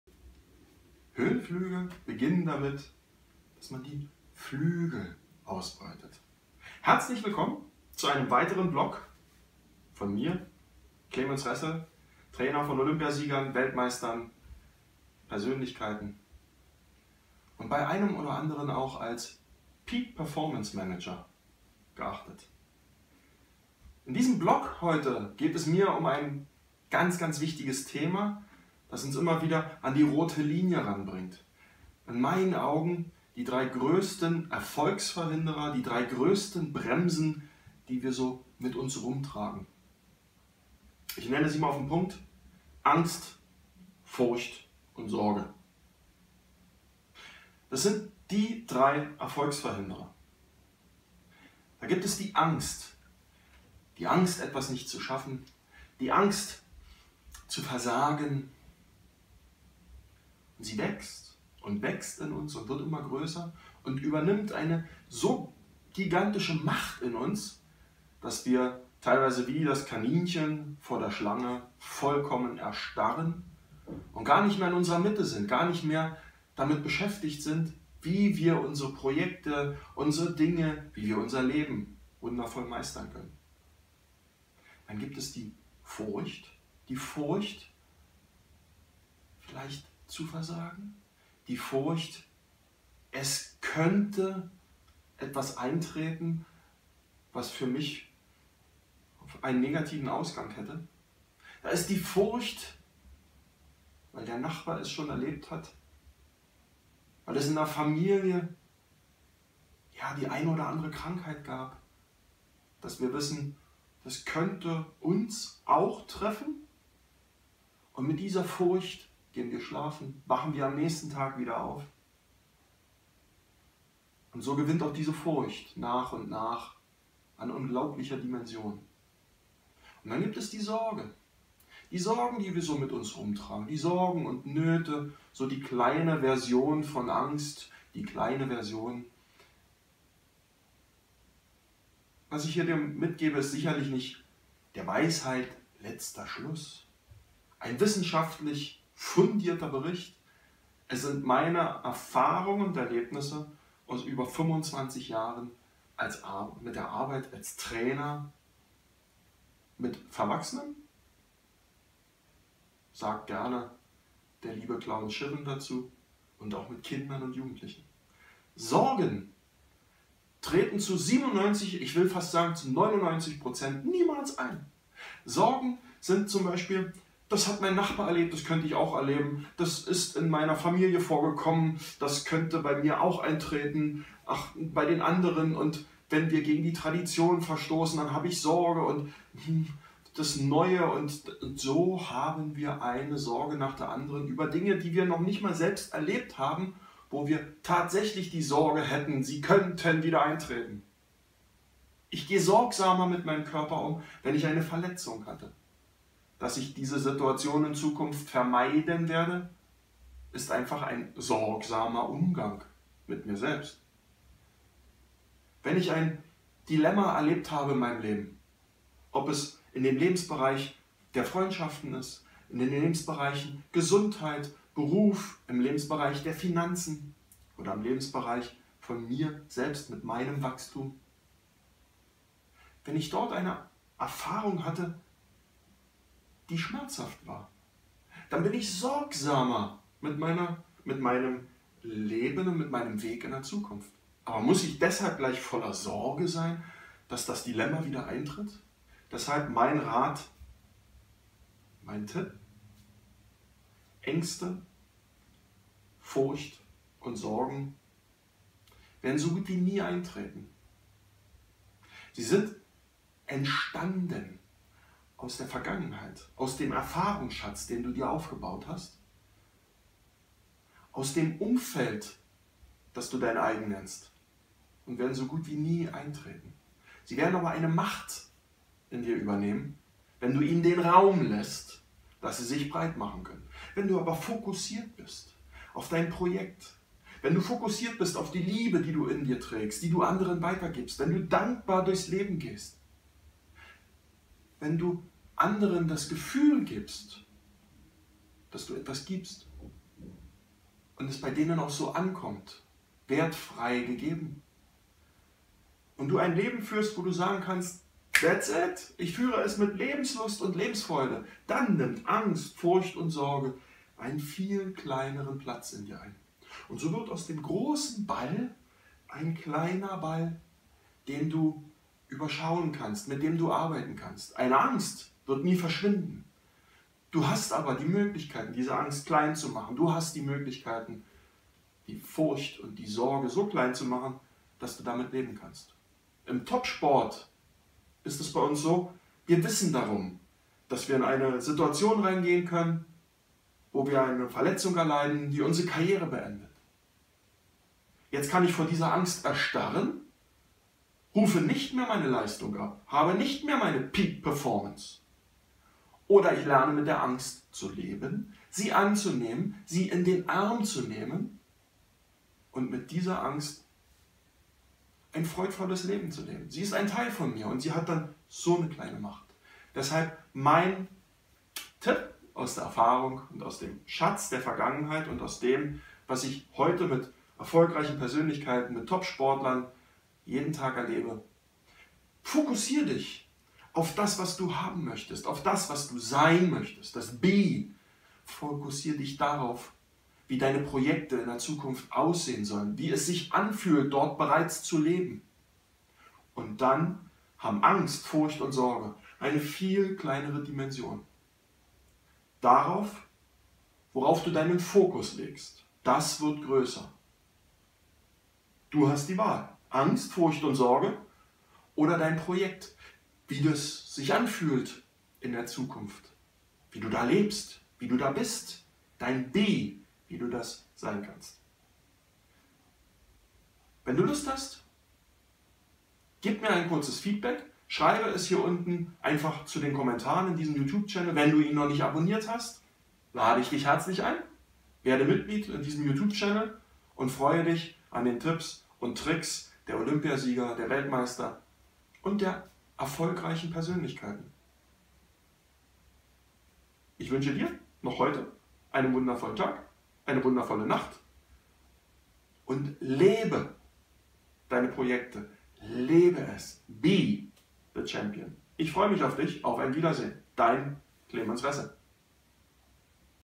Was verhindert Deinen Erfolg?
) In diesem Video erfährst Du welche Dinge Deinem Erfolg im Weg stehen. Die drei größten Erfolgsverhinderer und wie Du damit umgehen kannst.

Das Original-Video stammt von "Clemens Ressel" und wurde durch eine Creative Commons Lizenz (CC-BY) für die Wiederverwendung zugelassen.